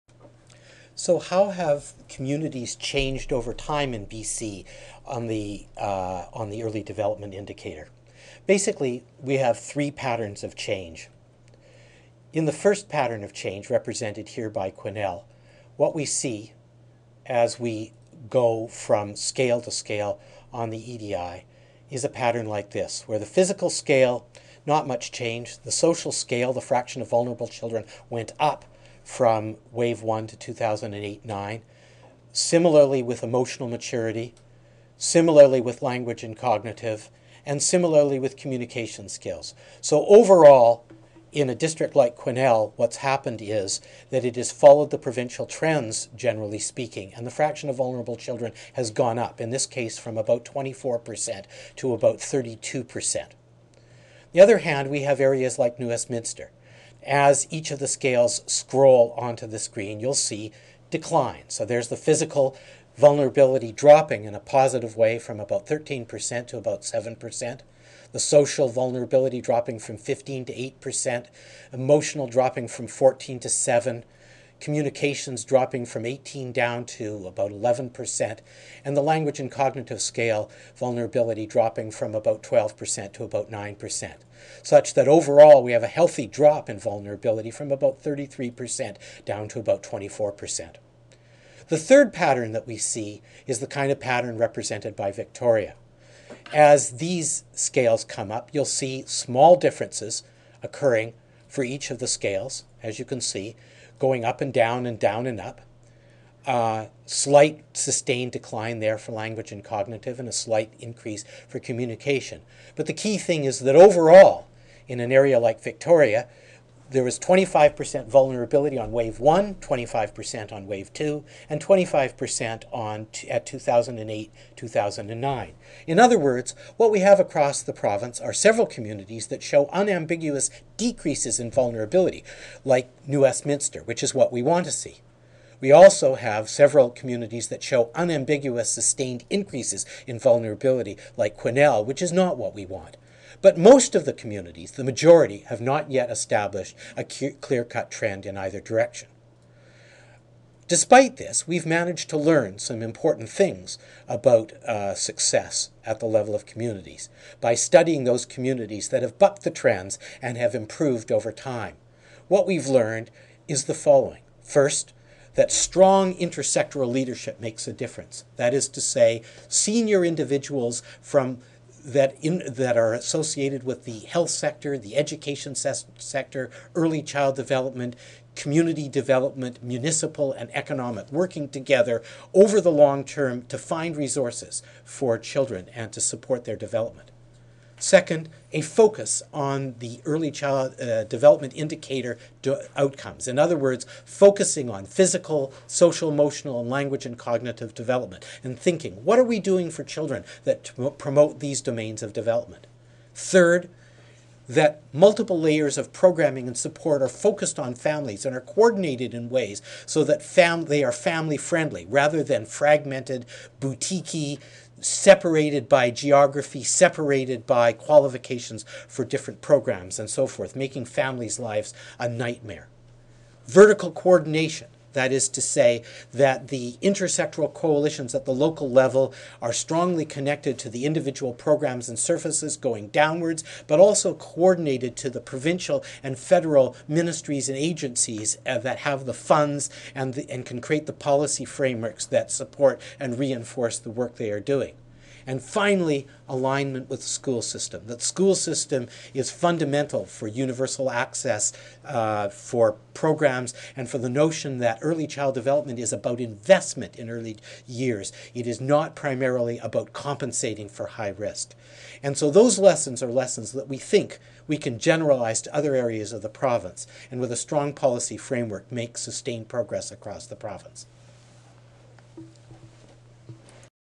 Patterns of EDI Change in BC
Dr. Clyde Hertzman, Director of the Human Early Learning Partnership describes patterns of change in Communities across BC based on data collected by the Early Development Instrument (EDI)